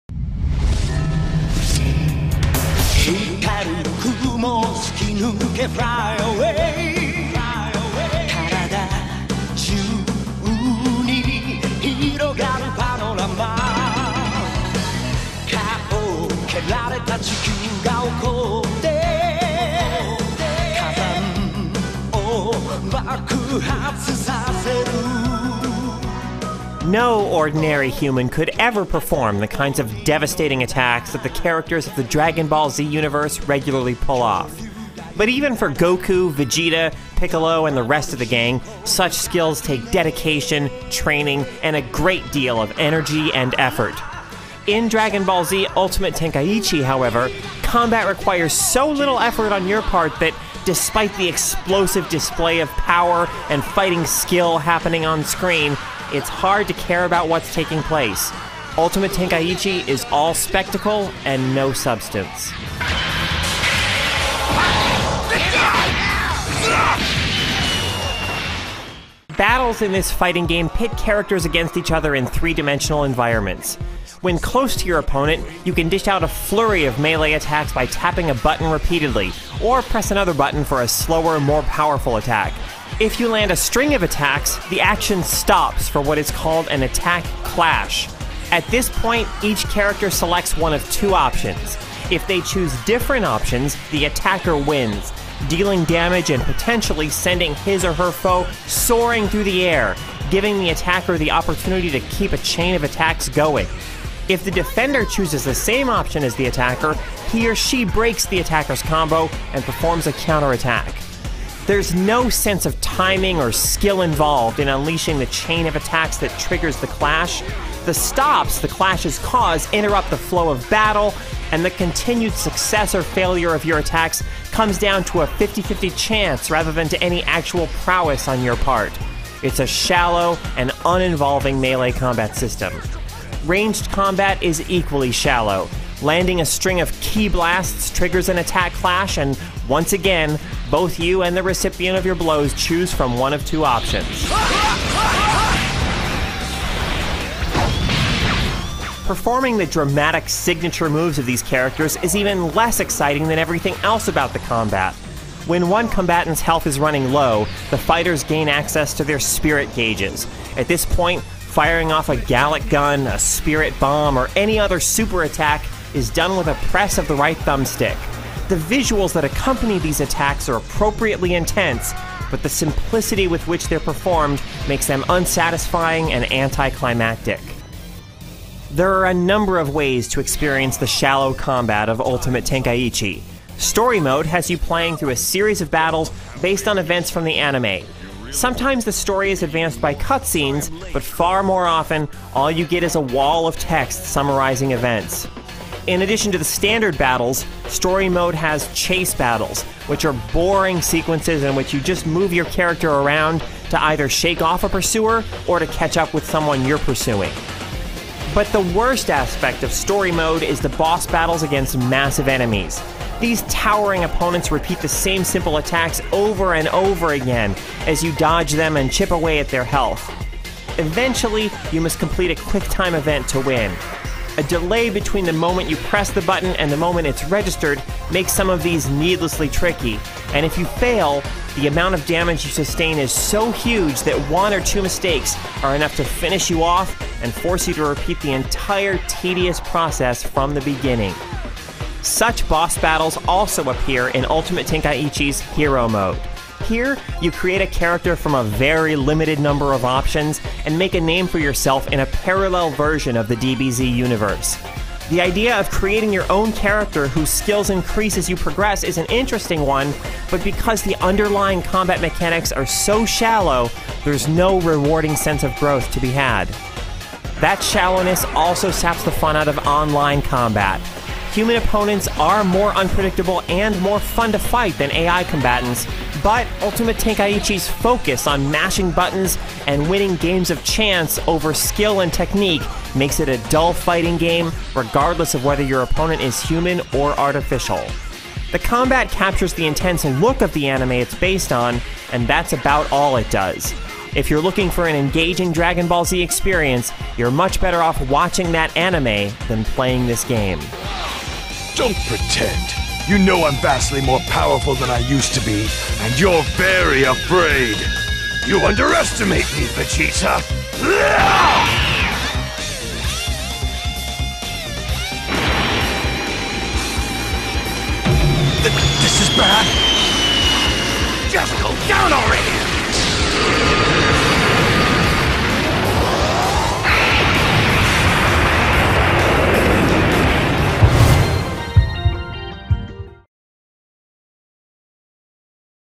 Dragon Ball Z: Ultimate Tenkaichi Video Review
Carolyn Petit charges up her special attack for this review of Dragon Ball Z: Ultimate Tenkaichi.